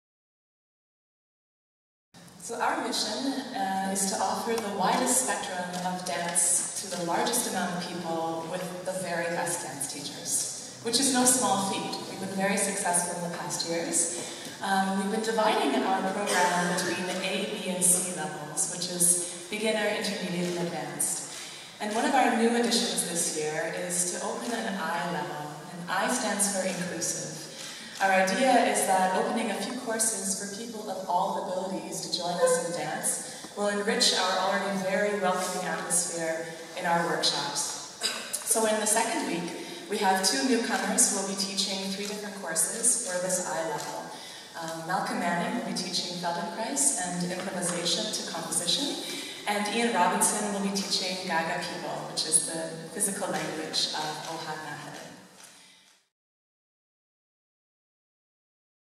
Sharon Booth about the new "I"-Level of Bolzano Danza WORKSHOPS
“I” stands for “Inclusive” which we define as dance activities which are designed to meet the needs of everyone in the class, regardless of ability, with a focus on exploring and developing individual creativity and physical potential.